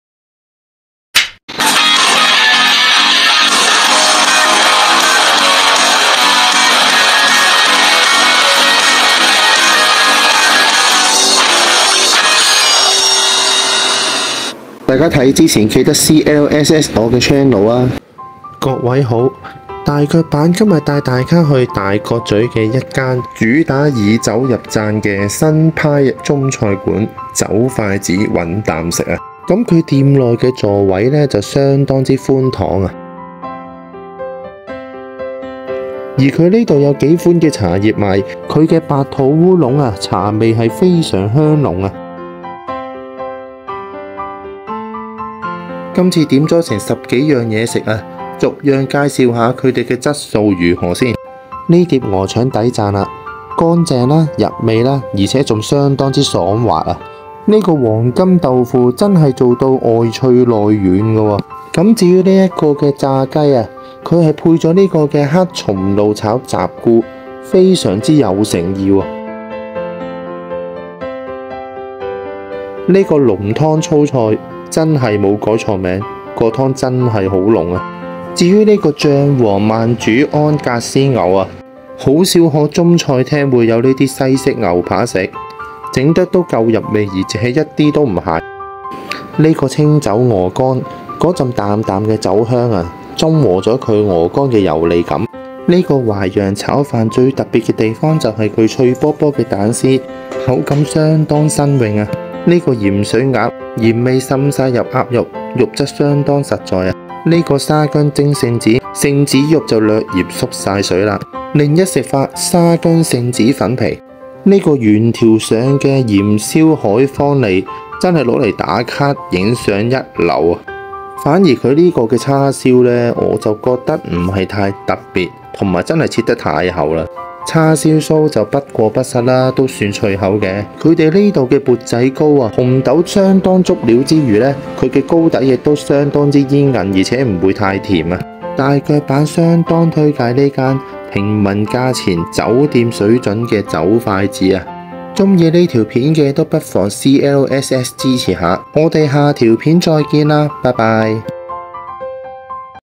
大腳板搵啖食 | 酒筷子 | 平民價錢酒店味道的新派中式粵菜館
不以裝橫招來
只靠味道吸引